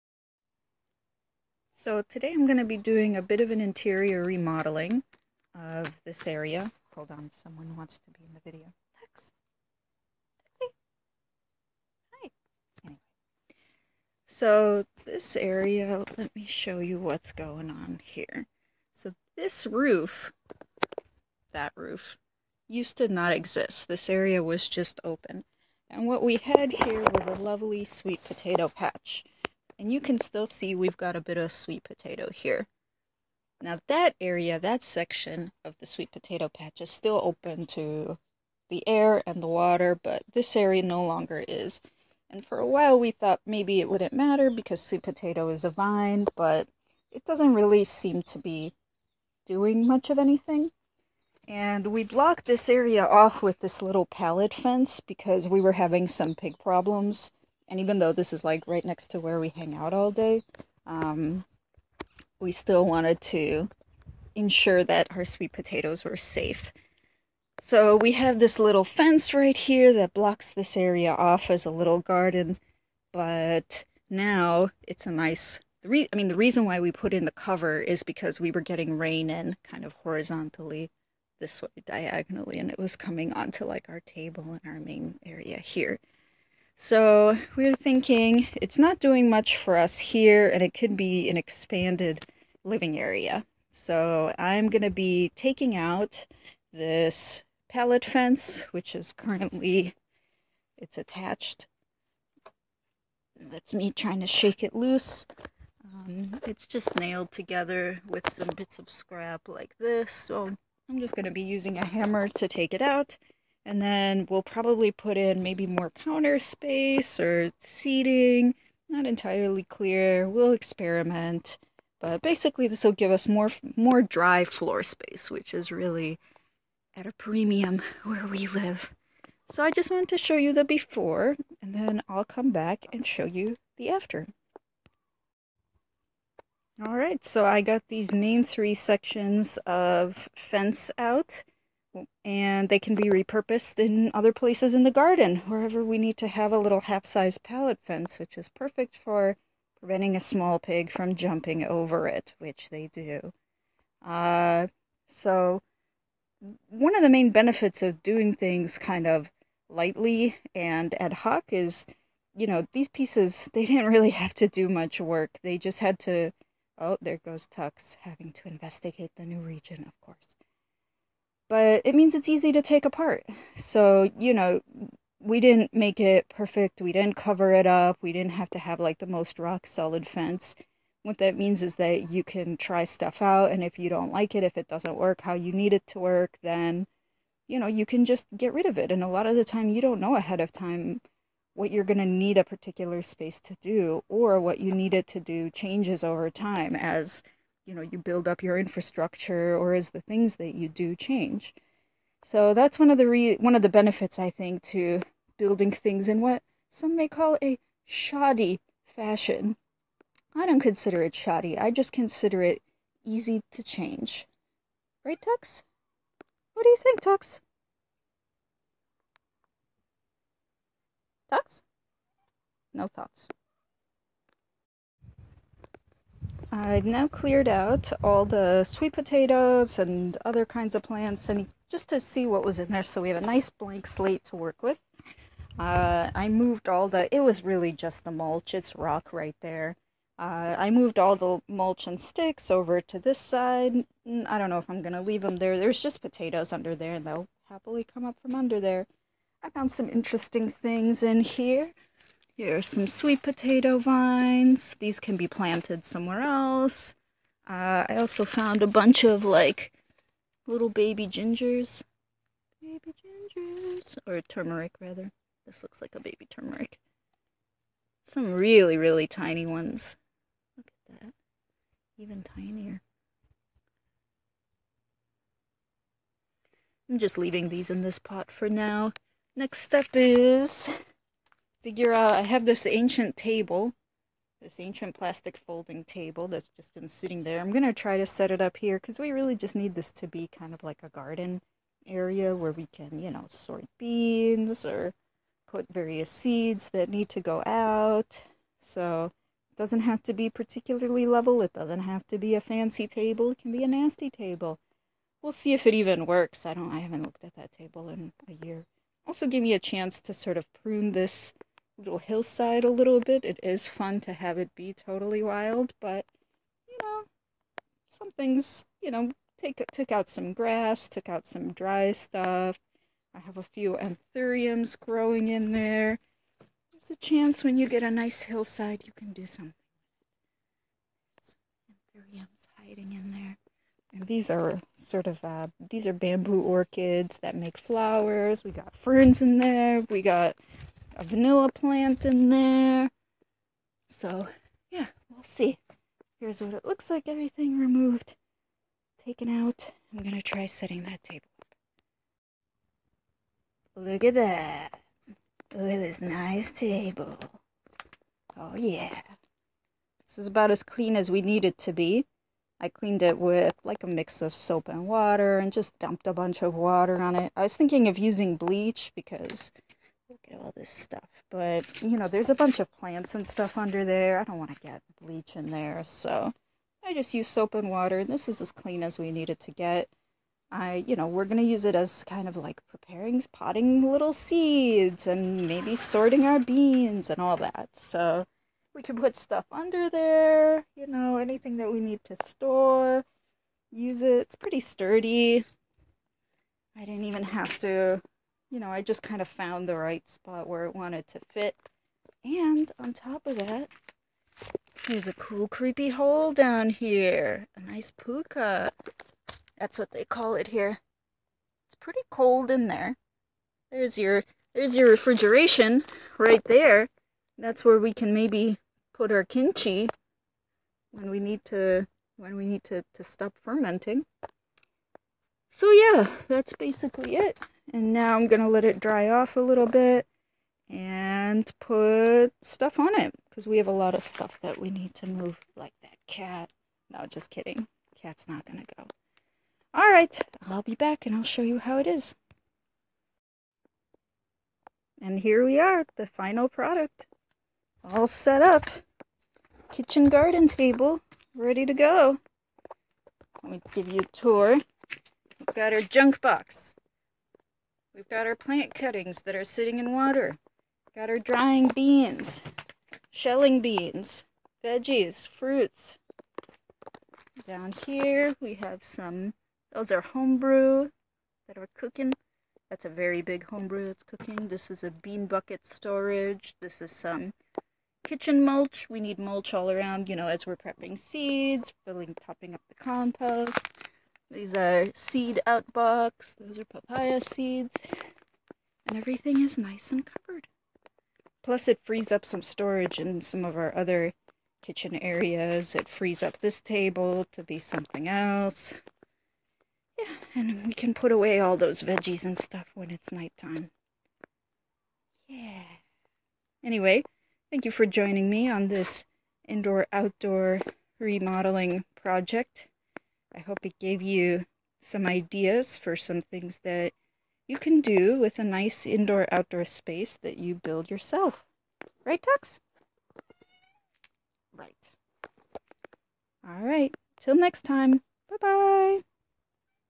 morning project: "remodeling" & expanding kitchen/garden space
before and after of a small reconfiguration of our outdoor kitchen/garden space.